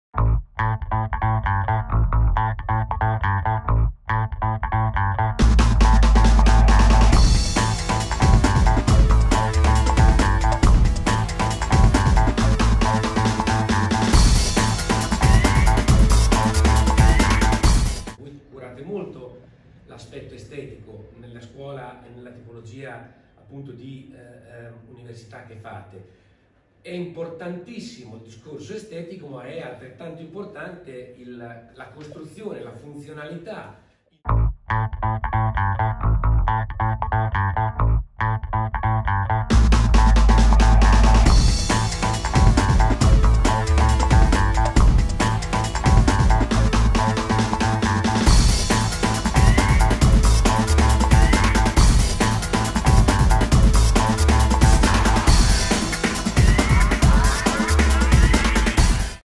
UNIVERSITÀ DEGLI STUDI DI NAPOLI “FEDERICO II”
OLTREMATERIA®  ALL’UNIVERSITÀ DEGLI STUDI DI NAPOLI “FEDERICO II”

Il Presidente di Oltremateria® , Sig. Loris Casalboni, è stato invitato il 25 Giugno 2021 presso la prestigiosa e antica Università degli studi di Napoli “Federico II” al Seminario Formativo

“ GREENBUILDING: Superfici continue decorative e architettura ecosostenibile”.

Il seminario, destinato principalmente a studenti e dottorandi a cui riconoscerà crediti formativi, ha visto anche la presenza di docenti, architetti e progettisti.